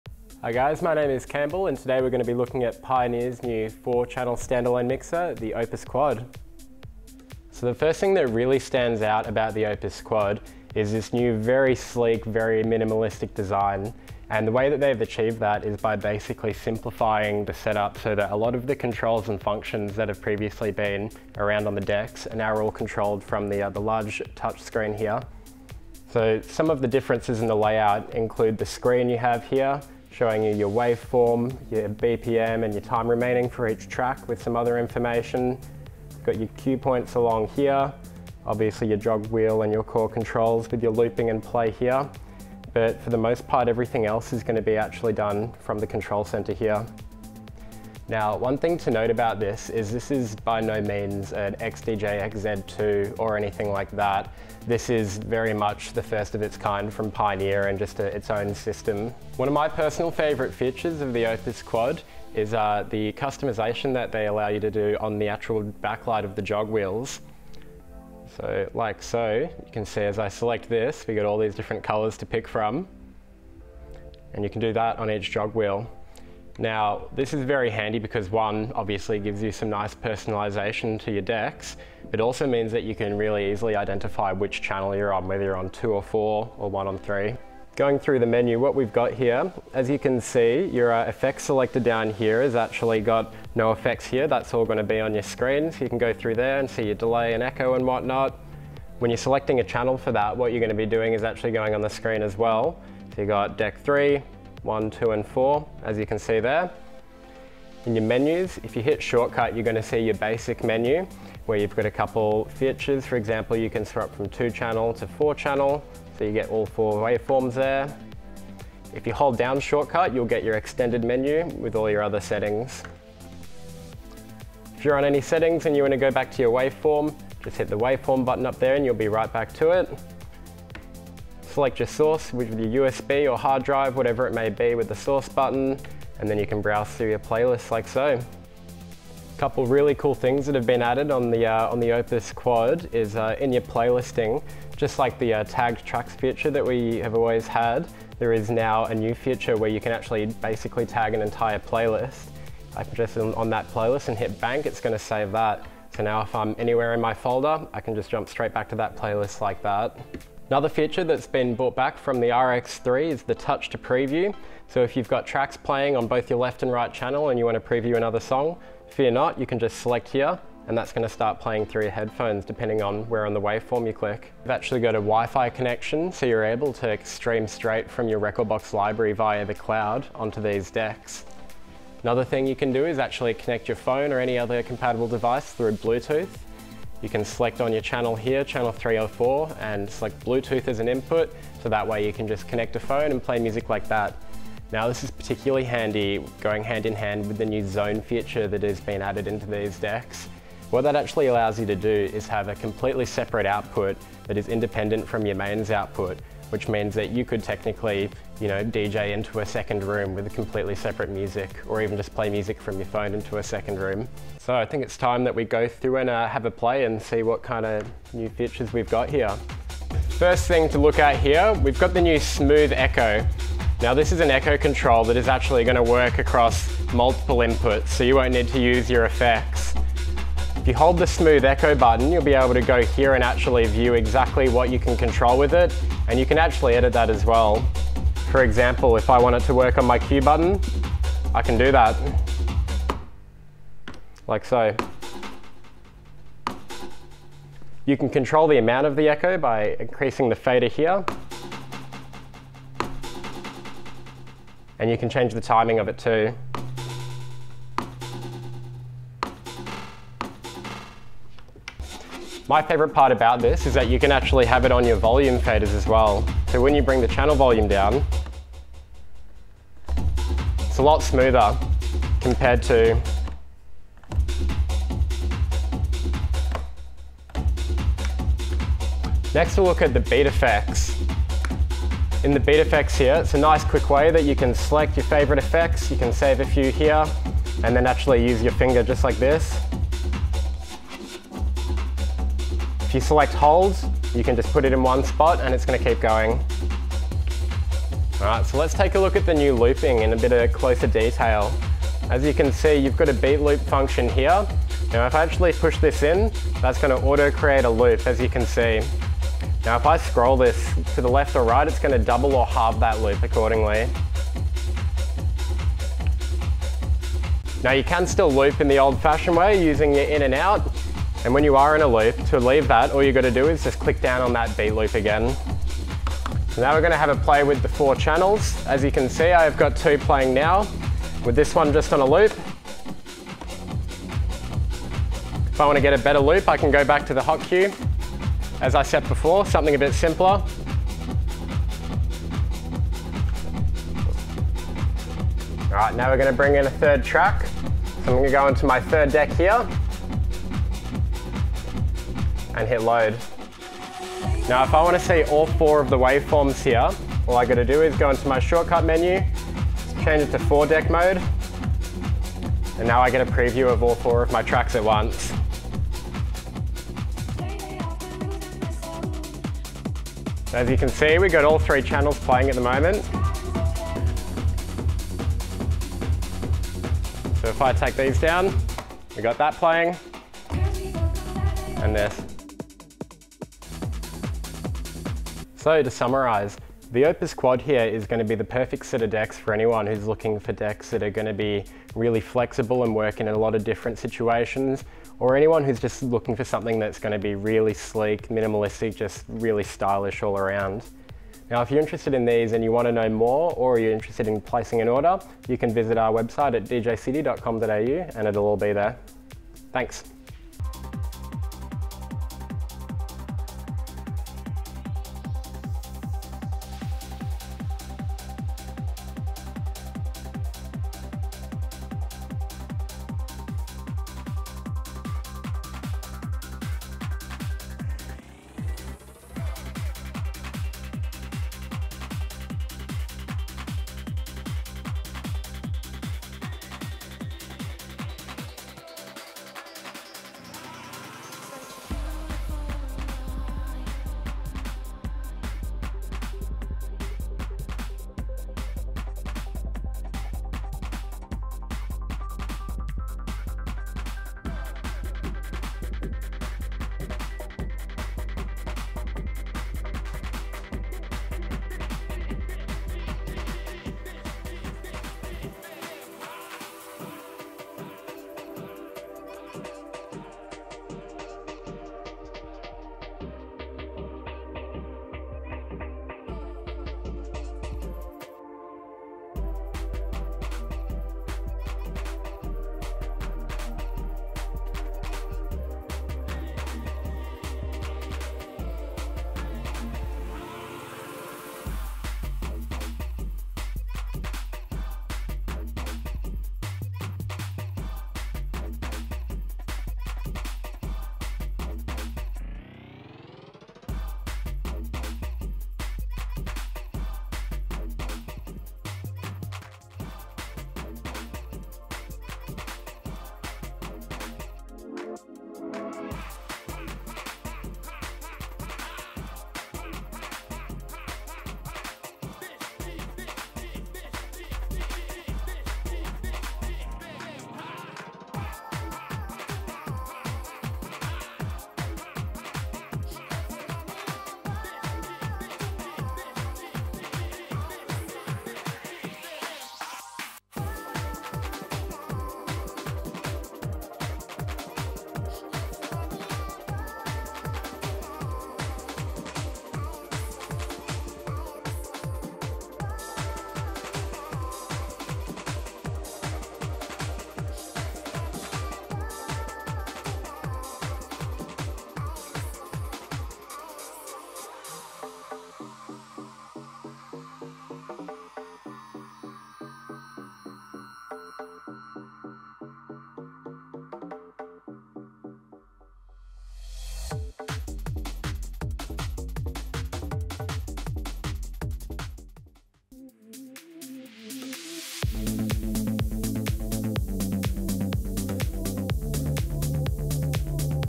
Pioneer Opus, Experience control like never before
A sleek design, versatile connectivity and customizable settings, The Opus is designed to meet your needs.